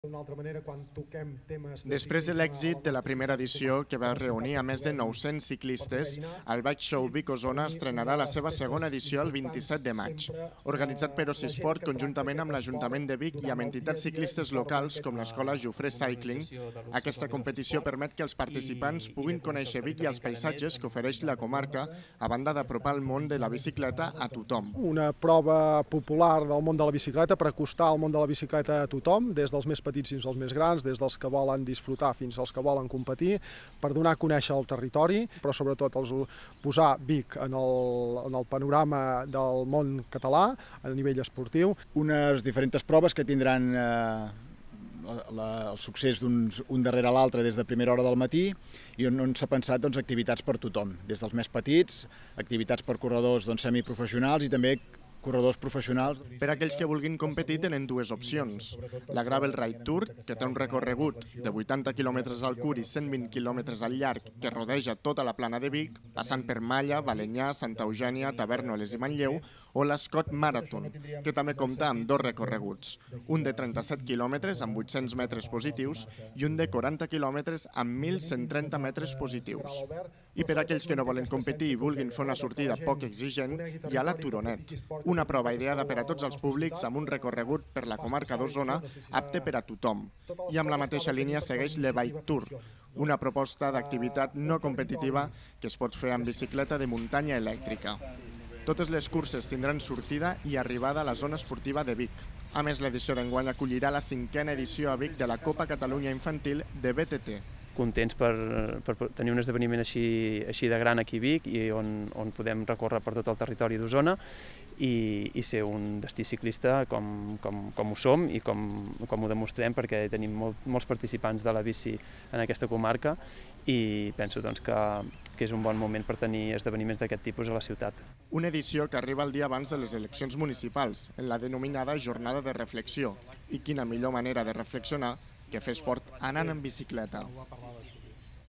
Vic acollirà la segona edició del Bike Show
Vic acollirà la segona edició de la Bike Show, després de l’èxit que va tenir l’any passat quan va reunir més de 900 ciclistes. Aquesta jornada està dirigida a tots els públics. És per això que hi ha curses per aquells que volen competir, com la Gravel Ride Tour o la Scott Marathon, o sortides poc exigents pensades per a famílies o pels que no vulguin competir, com La Turonet o l’E-Bike Tour. L’edició d’enguany també acollirà la cinquena edició a Vic de la Copa Catalunya Infantil de BTT.

INFORMA: Sergi Vives